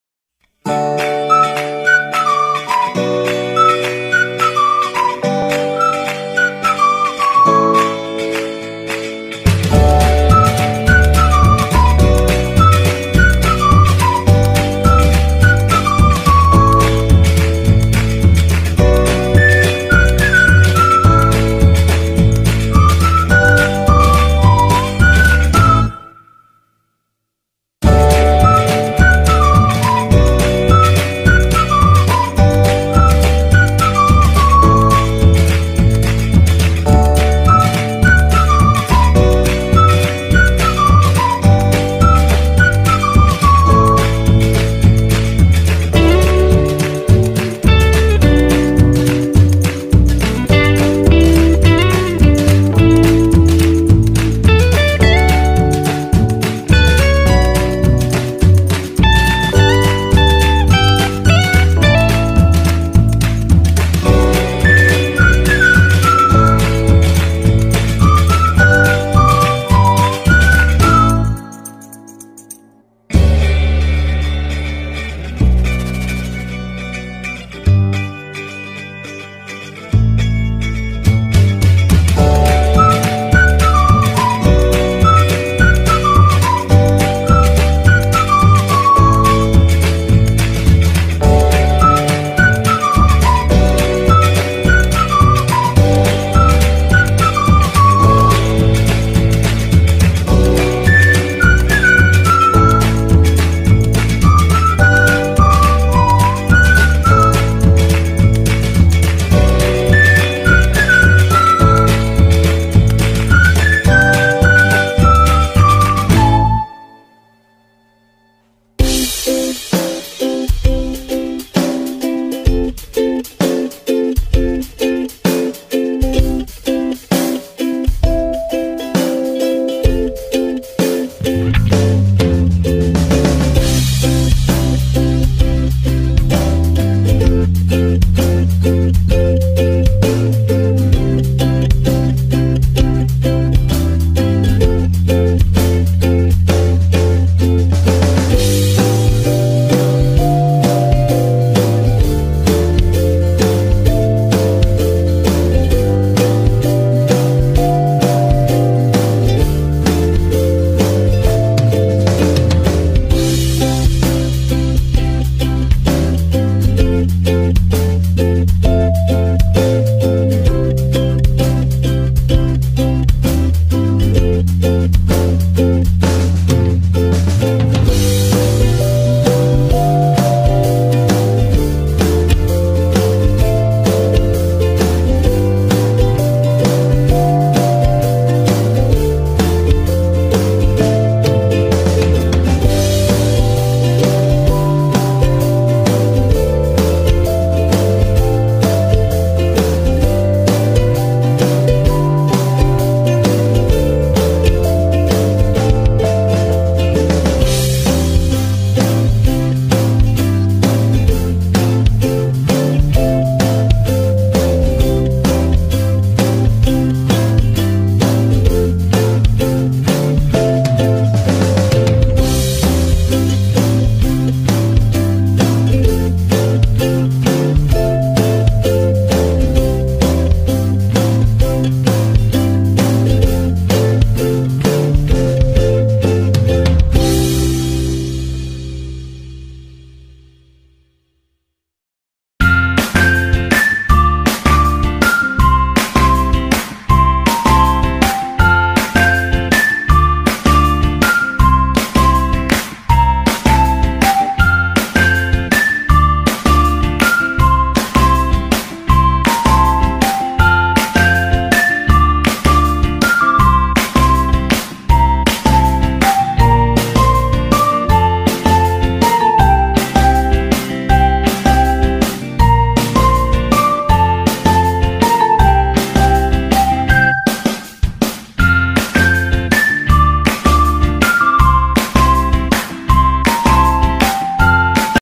Kağıttan Tilki Yapımı- Video 65
Herkese Merhabaa 15x15  ölçüleriyle tilki yaptım. Şimdiden kolay gelsin :)